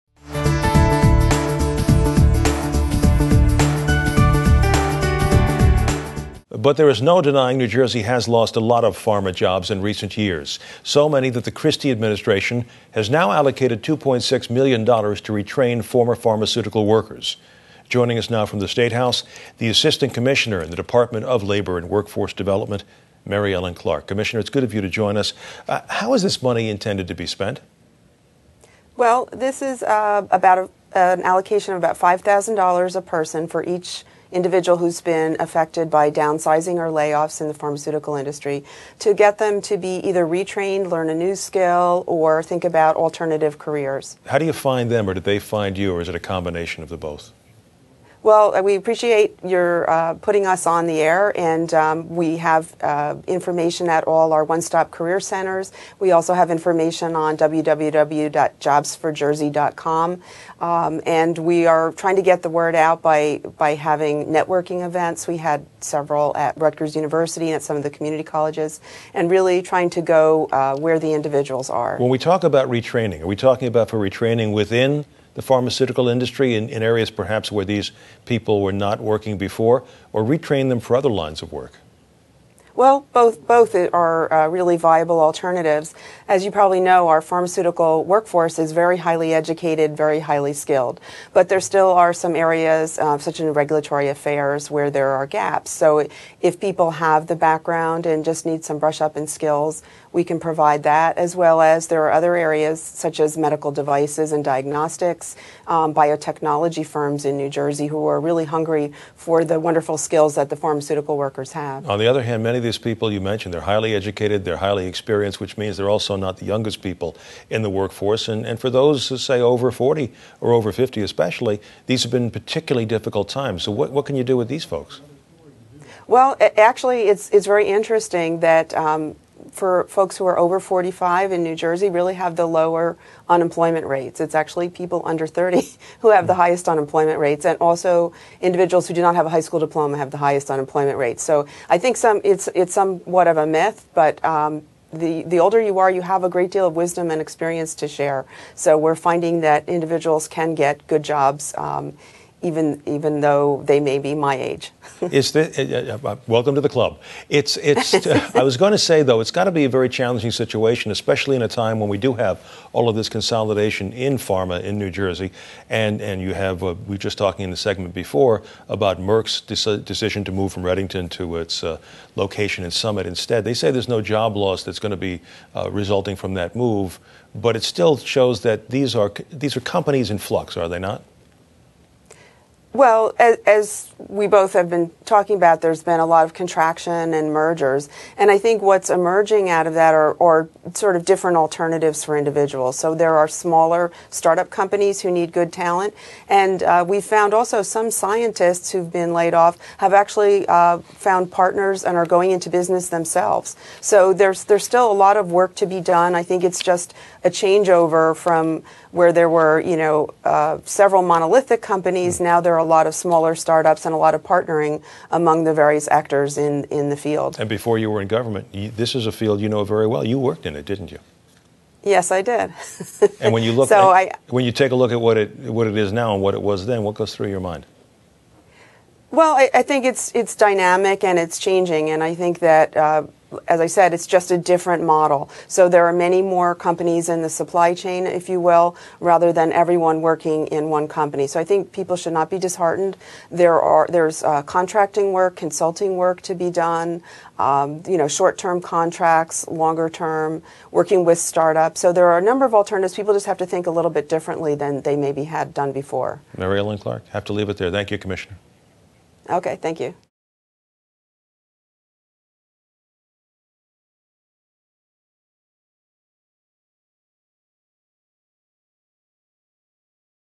Labor Department Seeks to Retrain Displaced Pharma Workers
The recent announcement by Merck that it plans to move its global headquarters — where 1,000 employees work — from Whitehouse Station to Summit, is the latest shake-up in the pharmaceutical industry which was once an economic stalwart in the Garden State. But mergers have cost New Jersey a lot of pharma jobs, which has led the Christie administration to allocate $2.6 million to retrain former pharmaceutical workers. NJ Today Managing Editor Mike Schneider spoke with Mary Ellen Clark, Assistant Commissioner of Labor and Workforce Development (LWD), about the initiative. For more New Jersey news, visit NJ Today